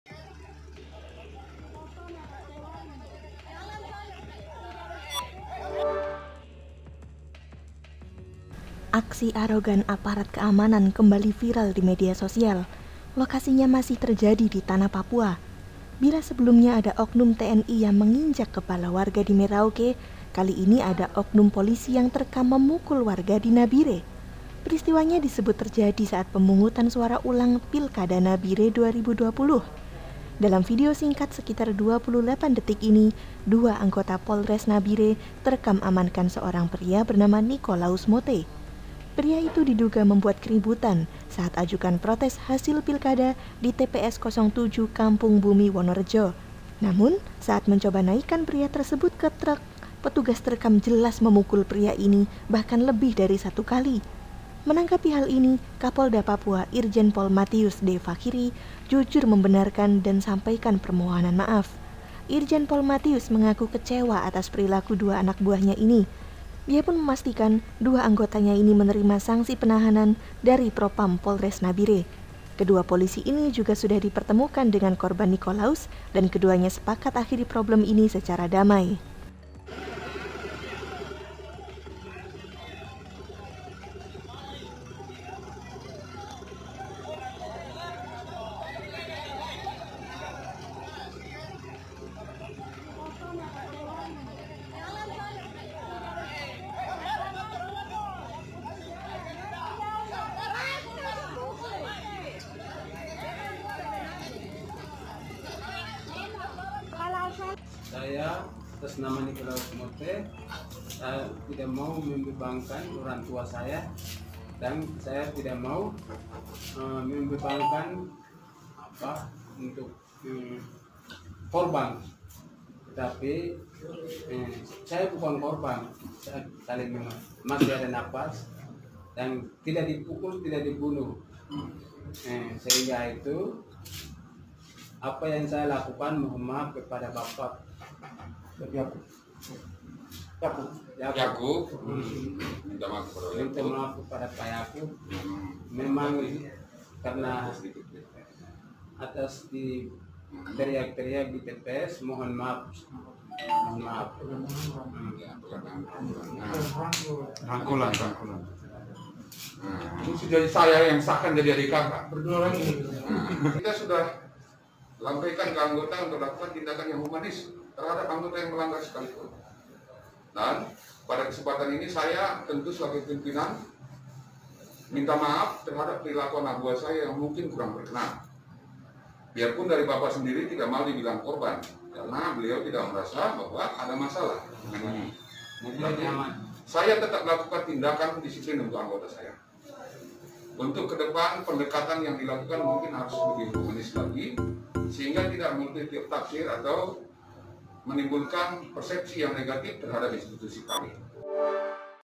Video Viral Polisi Pukul Kepala Warga, Polda Pastikan Pelaku Disanksi
TRIBUNJATIM.COM, JAKARTA -

Bila sebelumnya ada oknum TNI yang menginjak kepala warga di Merauke, kali ini ada oknum polisi yang terekam memukul warga di Nabire

Peristiwanya disebut terjadi saat pemungutan suara ulang Pilkada Nabire 2020.

Dalam video singkat sekitar 28 detik ini, dua anggota Polres Nabire terekam amankan seorang pria bernama Nicolaus Mote

Sumber: Kompas
Sumber Video:
- Fabeook Natalis Bunay